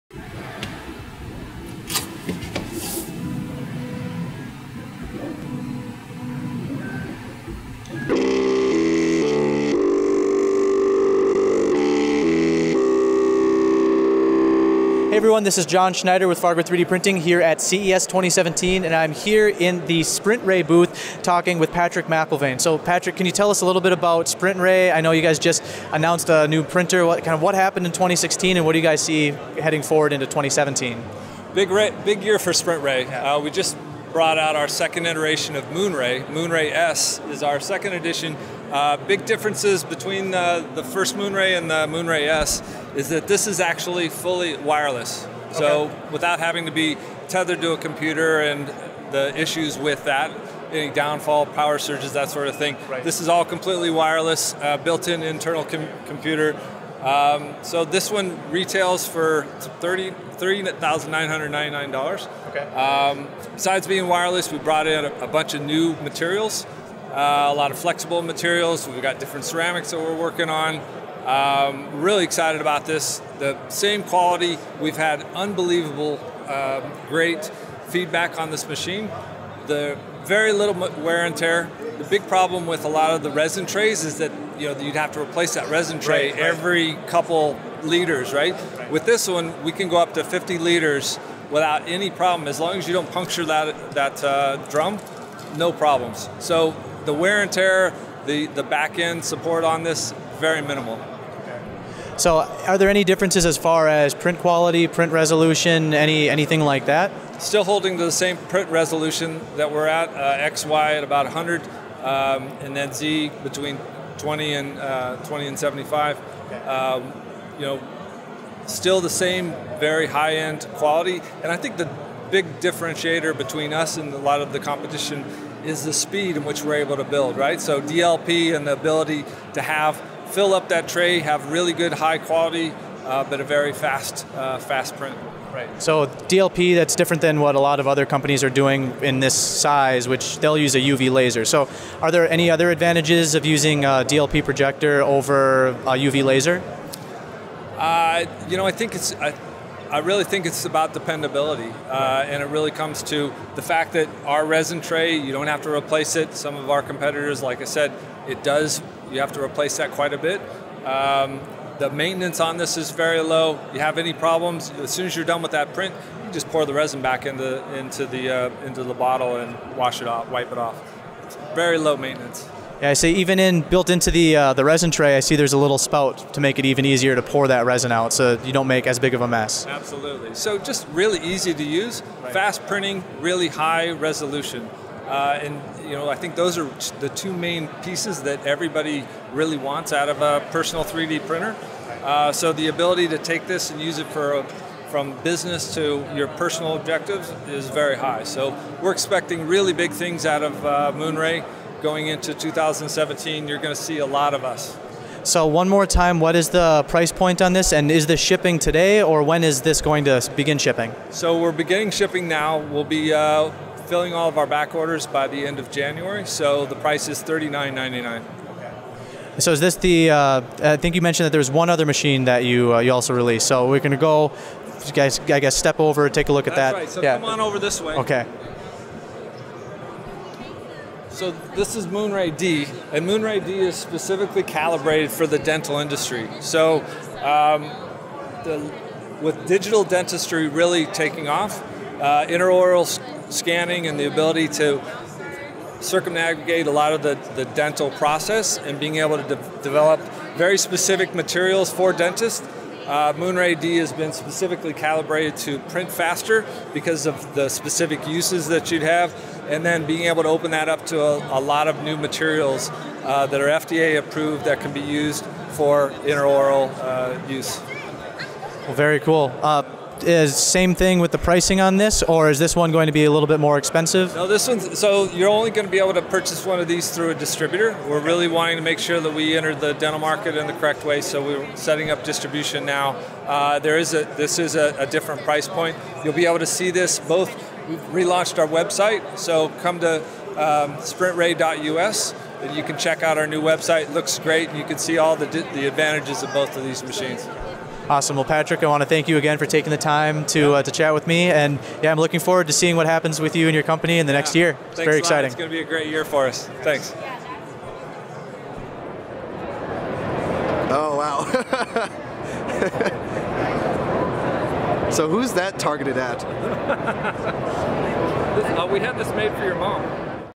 High Speed DLP Printer - SprintRay | CES 2017
SprintRay has released its second iteration of the MoonRay printer. The MoonRay S is wireless so it doesn't need to be tethered and has a small internal computer. Unlike many resin printers, it uses DLP technology instead of a laser. This helps with the speed and dependability of prints.

New materials are in the works including flexible and ceramic. Each resin tray will last up to 50L and features a spout to allow for easy cleanup.

A dental version, called MoonRay D, is now available. To ensure a proper entrance to the dental industry, it will only be available through distributors.